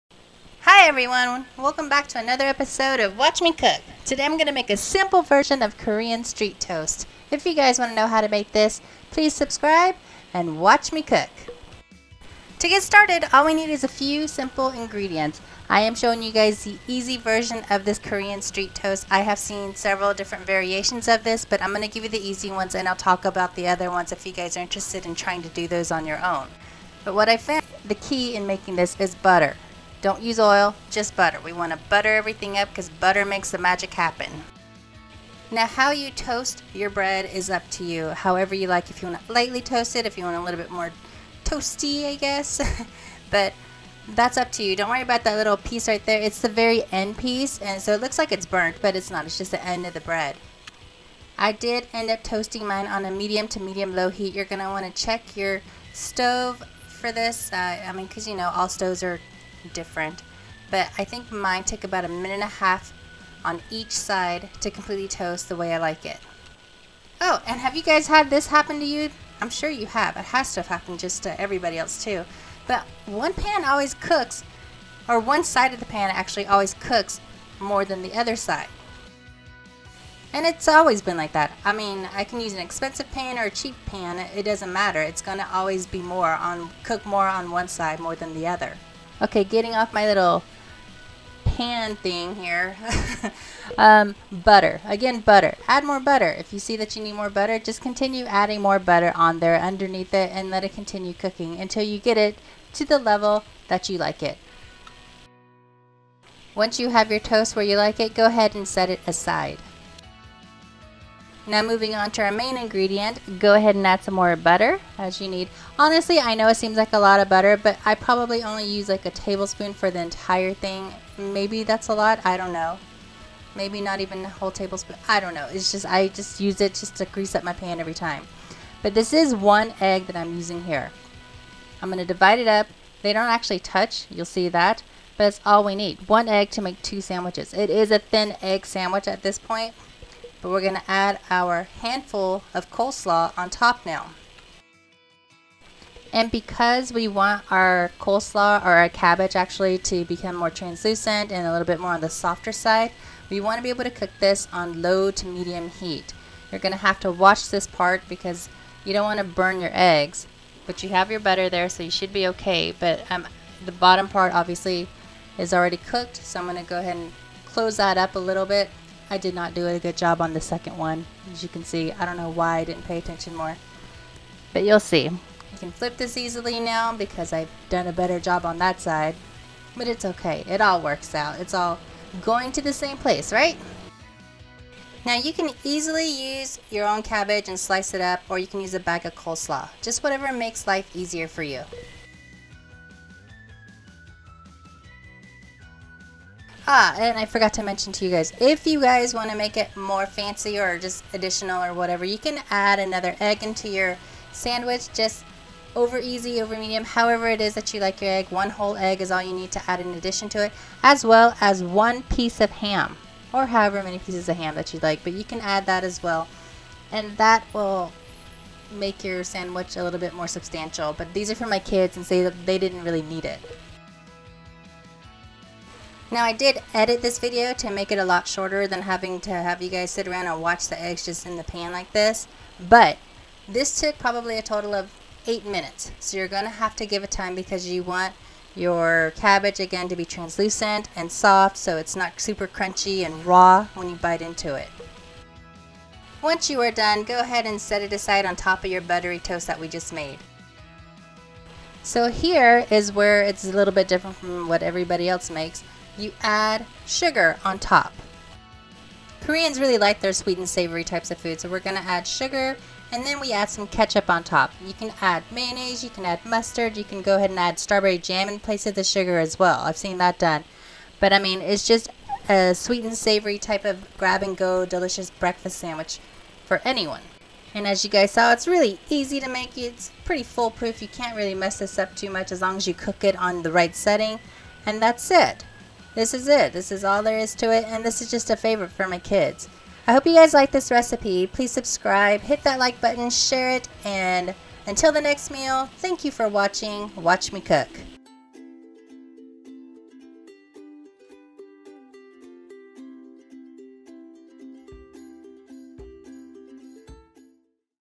Easy Korean Street Toast WatchMiCook Episode 75
I watch a lot of Korean Shows and I see a lot of food stalls with street foods but I live here in SoCal so going to a corner street food vendor anytime I want in Korea is not an option. So, let's make a super simple Korean Street Toast Sandwich! This is an easy to make breakfast sandwich thats just ready to go and eat!

Koreans love their veggies, sweet and savory. So this has it all. Buttery eggs, toast with some cabbage, sugar and ketchup.

Optional add ins are strawberry jam, ham, an additional egg, cheese, etc.

The beauty about cooking is you don't have to just follow step by step. You can always be more creative and make foods to suit you!

This is just a basic and easy street toast recipe and you can be as creative as you want!

Use your substitutions as needed to keep this diabetic friendly and gluten free!

WatchMiCook

or